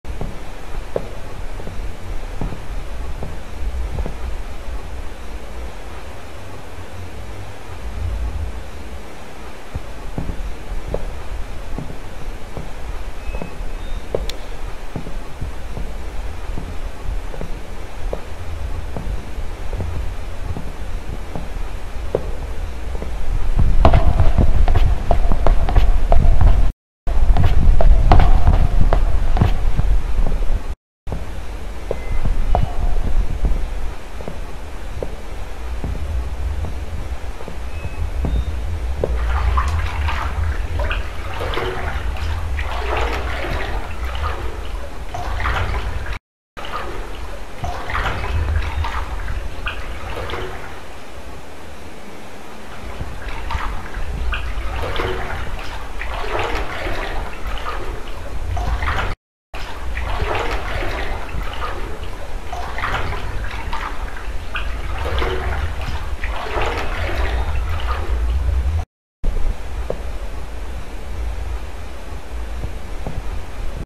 Luckily the Poolrooms are safe! | Backrooms Found Footage #1
My Uncle said he was trapped in an Indoor Swimming pool and sent me this video.